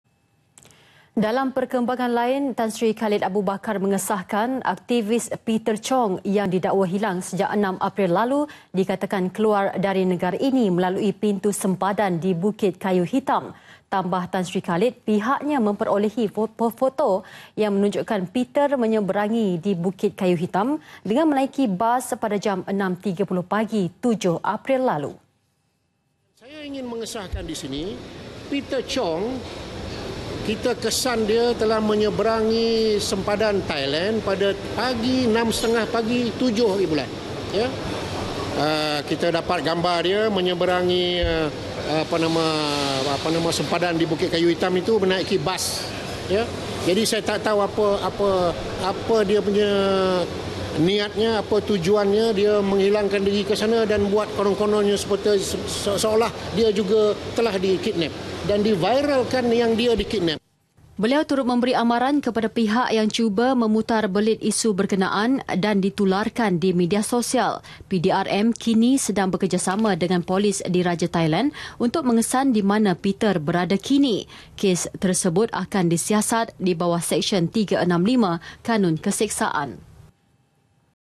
IGP: PETER CHONG DILIHAT SEBERANG SEMPADAN THAILAND [12 APRL 2017]
Dalam perkembangan lain... Tan Sri Khalid Abu Bakar mengesahkan  aktivis Peter Chong yang didakwa hilang sejak 6 April lalu.. , dikatakan keluar dari negara ini  melalui  pintu sempadan di Bukit Kayu Hitam.

Tambah Tan Sri Khalid... pihaknya memperolehi  foto yang menunjukkan Peter menyeberangi  di Bukit Kayu Hitam dengan menaiki bas pada jam 6.30 pagi 7 April lalu.